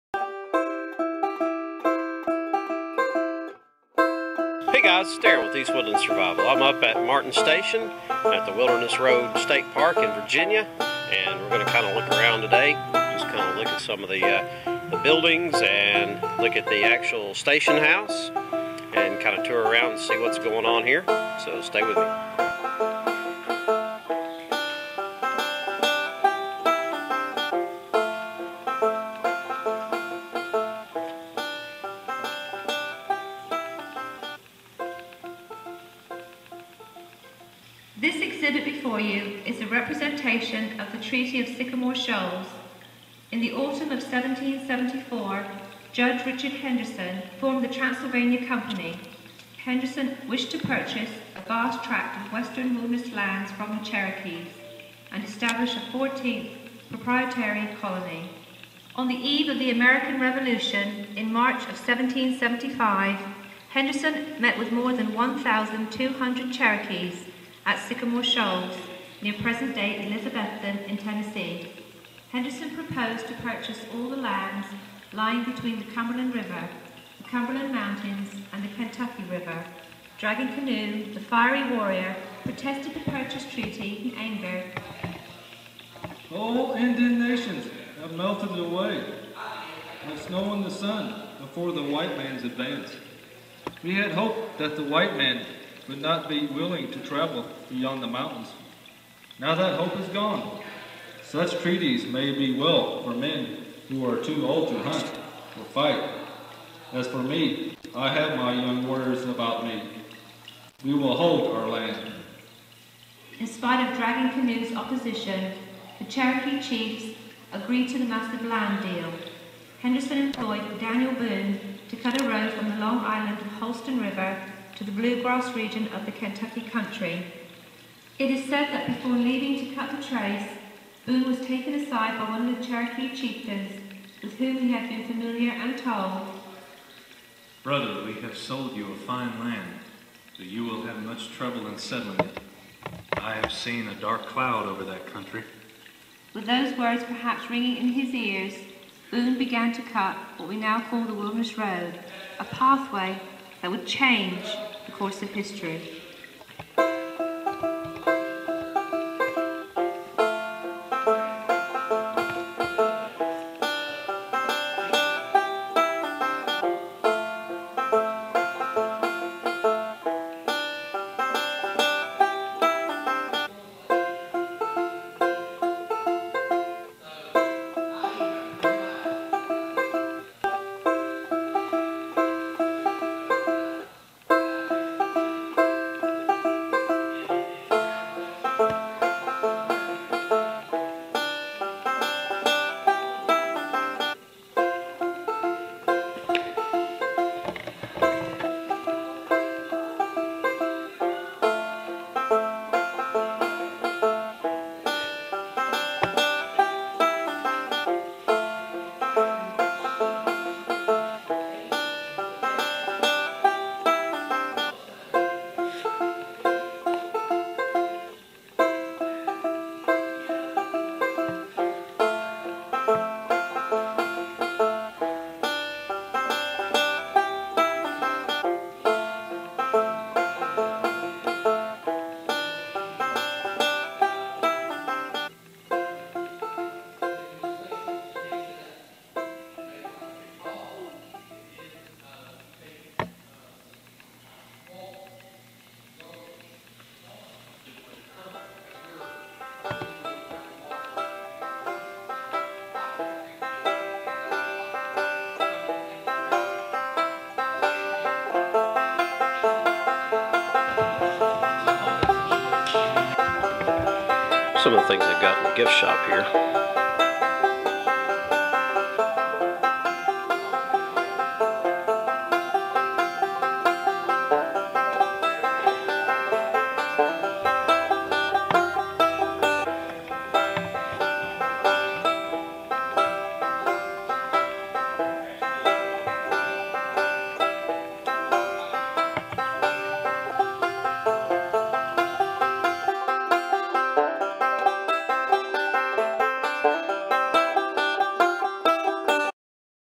Martin's Station Museum
A tour of the Martin's Station Museum at the Wilderness Road State Park in Ewing, VA.  Thanks to the staff at the park for allowing me to film.